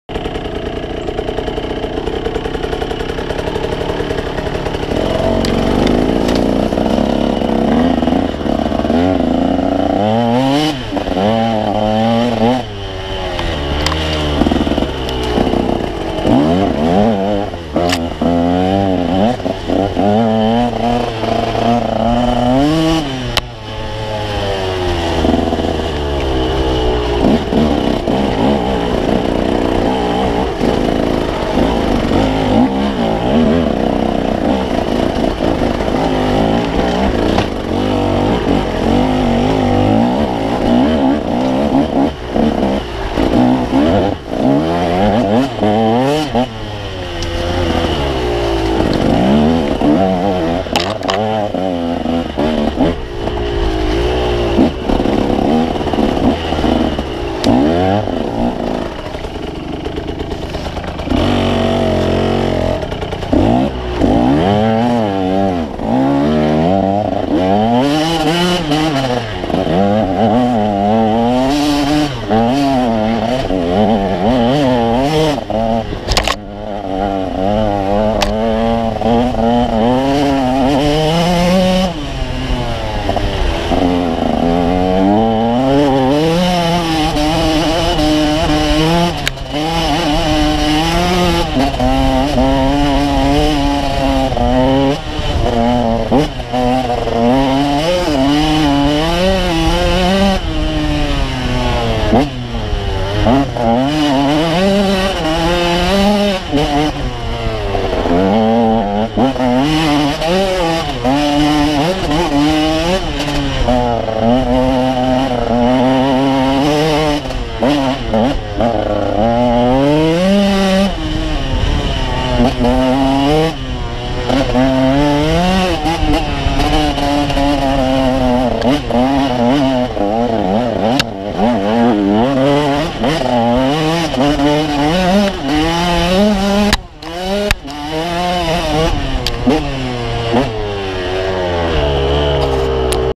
Cambell's Creek Fun on the 150
Slick creek at 8mile, fun section up to the ridge on my way home.